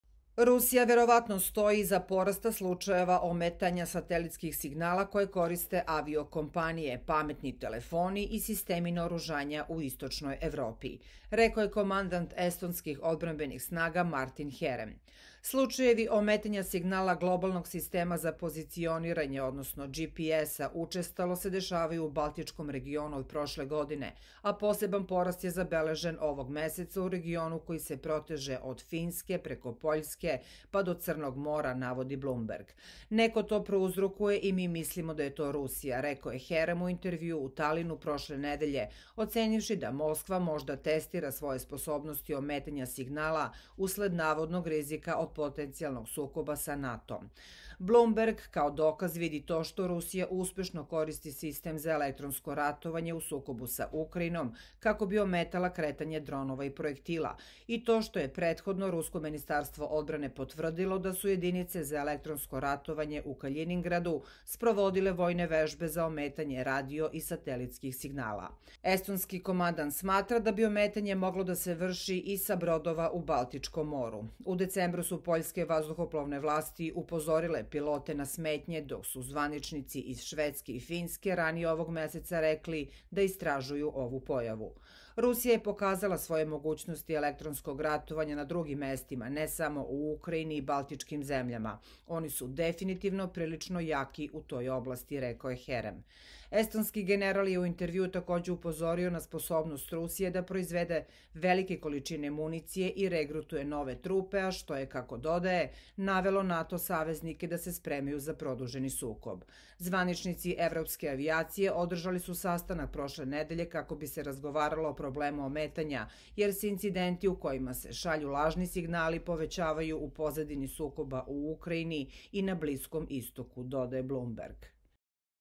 NATO u panici: Rusi ometaju GPS i satelitski signal nad Evropom
Rusija verovatno stoji iza porasta slučajeva ometanja satelitskih signala koje koriste aviokompanije, pametni telefoni i sistemi naoružanja u istočnoj Evropi, rekao je komandant estonskih odbrambenih snaga Martin Herem.